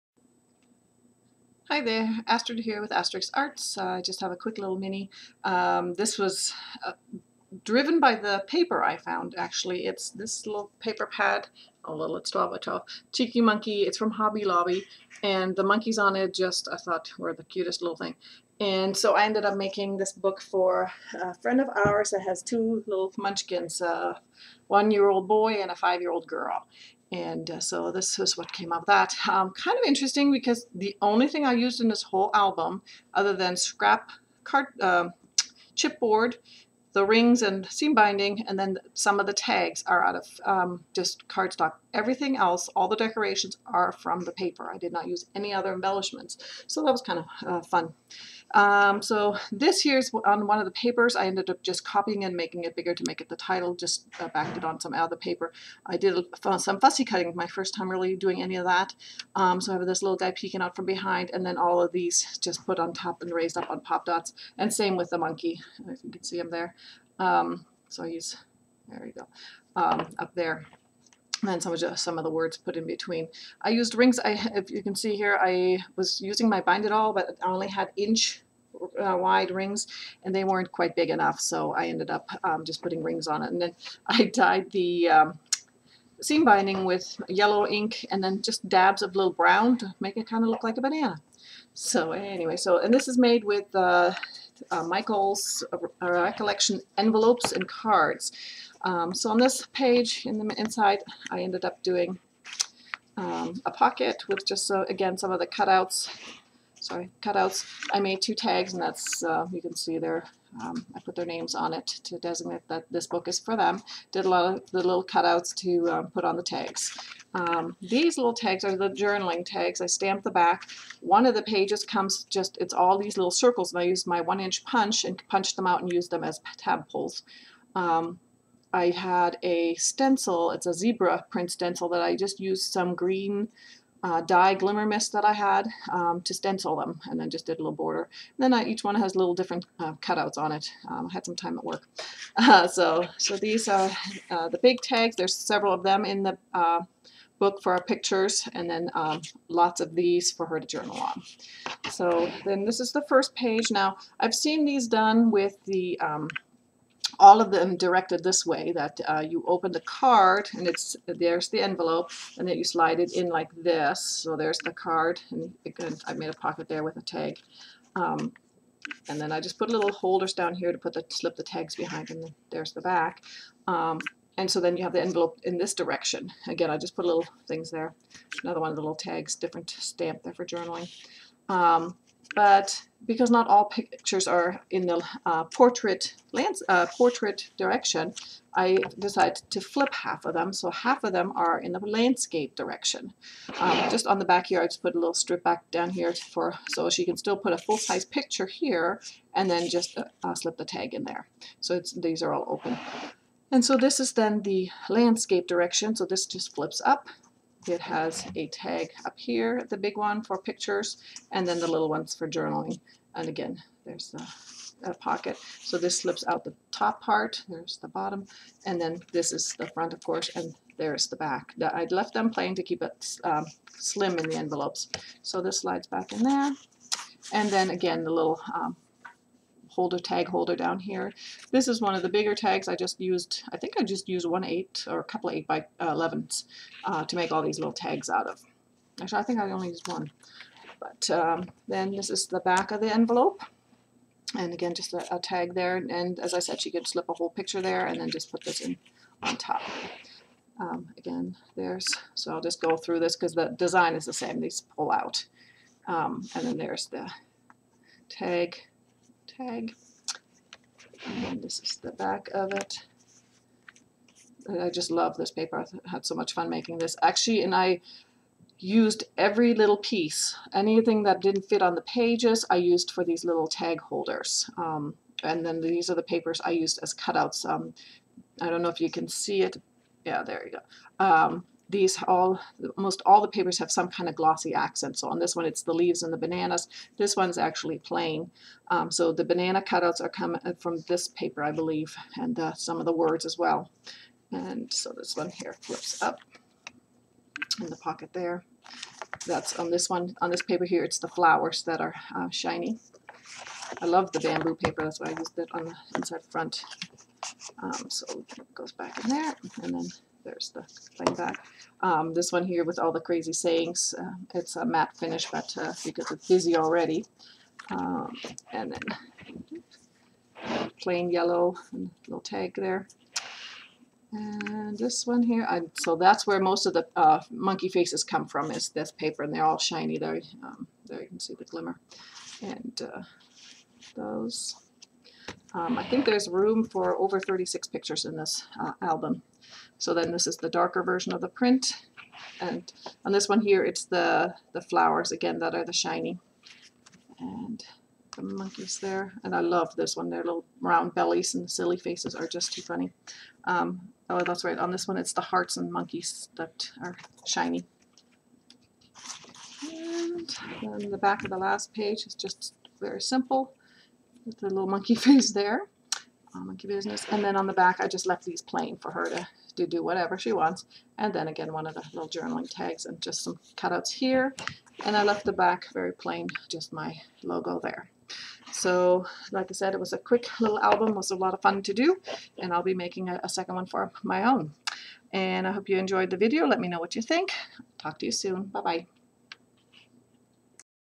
Monkey Business Mini Album
A mini album made of Recollections cards and envelopes, using Paper Studios Cheekee Monkey paper stack.